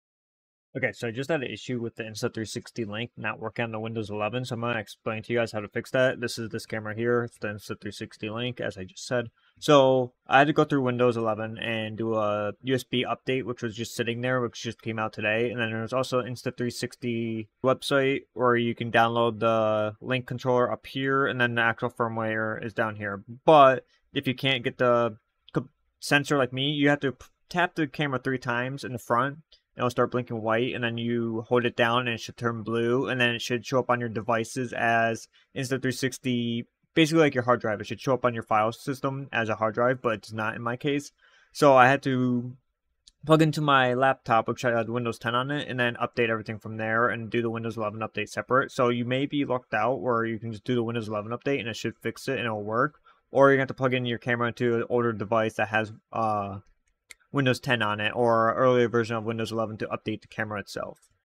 insta 360 link issues
How to fix your insta360 camera if it is not working on your computer.